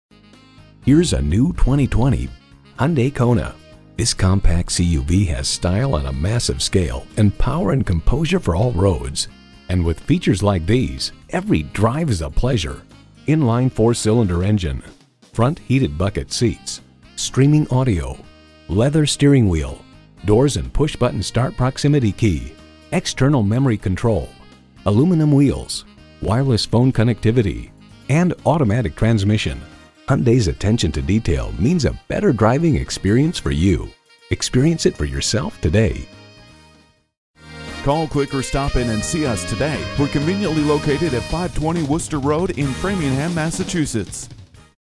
New 2020 Hyundai Kona Framingham, MA #17712 - SOLD
Year: 2020
Make: Hyundai
Model: Kona
Trim: SEL
Engine: I4
Transmission: Automatic
Color: Blue
Interior: Black
Stock #: 17712
VIN: KM8K2CAA8LU545948
Automatic All-Wheel Drive System, 2.0L Atkinson Cycle 4-Cylinder Engine, 6-Speed Automatic Transmission with Drive Mode Select, Forward Collision-Avoidance Assist, Lane Keeping Assist and Driver Attention Warning, Blind-Spot and Rear Cross-Traffic Collision Warning, Multilink Rear Suspension, Downhill Brake Control, Hillstart Assist Control, Front, Front Side & Side-Curtain Airbags w/ Rollover Sensors, Tire Pressure Monitoring System w/ Individual Tire Indicator, BluetoothA® Hands-Free Phone System, 7-inch Color Touchscreen Audio Display with Rear View Monitor, Android Auto(TM) & Apple CarPlay(TM), AM/FM/MP3/HD RadioA® Audio System w/Aux & Dual USB Outlets, SiriusXMA® Radio, Hidden Cargo-Area Storage Compartment, 17" Alloy Wheels, Automatic On/Off Headlights, LED Daytime Running Lights, Roof Side Rails, Variable Intermittent Windshield Wipers, Power Door Locks & Windows with Front Auto-Down/Up, Heated Side Mirrors with Turn Signal Indicators, Proximity Key with Push Button Start, Immobilizer, 6-way Driver seat with Height Adjustment, Heated Front Seats, 60/40 Split Folding Rear Seat w/ Center Armrest, Air Conditioning with Cabin Air Filter, Leather-Wrapped Steering Wheel and Shift Knob, Tilt-and-Telescopic Steering Wheel w/Audio, Cruise & Phone Controls, LCD Multi-Information Display, Carpeted Floor Mats and Roof Rack Cross Rails.

Address:
520 Worcester Rd - Rte 9 East